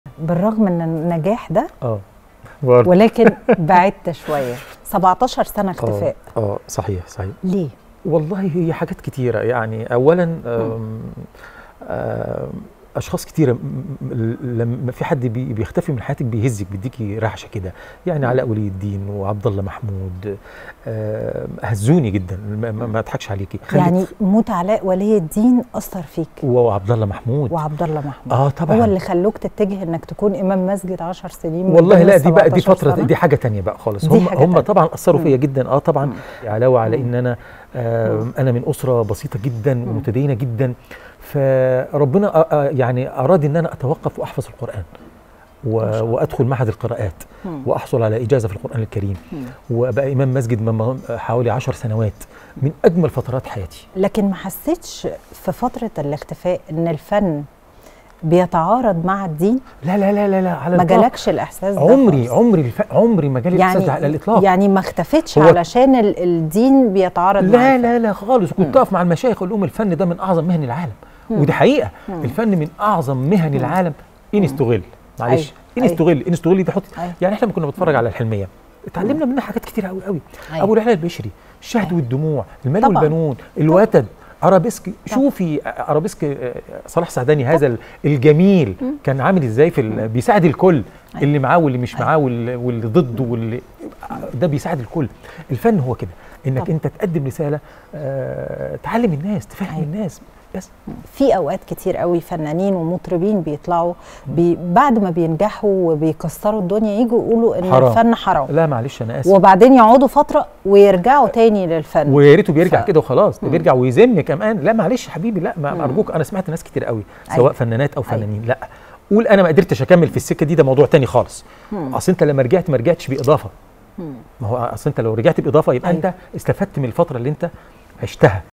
وقف في وجه المشايخ وقال "الفن من أعظم الفنون".. الفنان حسن فؤاد يكشف عن فترة إمامته لمسجد
وقف في وجه المشايخ وقال "الفن من أعظم الفنون".. الفنان حسن فؤاد يكشف عن إمامته لمسجد وإيجازته لقراءة القرآن الكريم